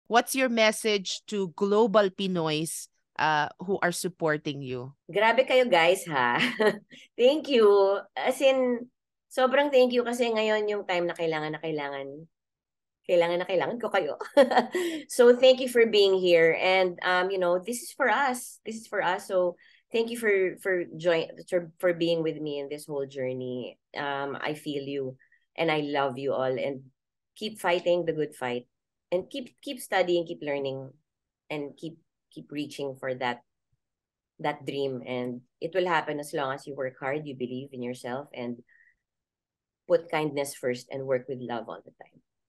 Dolly de Leon's message to global Pinoys
Fresh from the Golden Globe Awards, Dolly de Leon shares her message to global Pinoys who continue to express their love and support as she brings honor to the Philippines in the international stage. Watch out for her interview on "Stronger Together: The GMA Pinoy TV Podcast" this January 2023!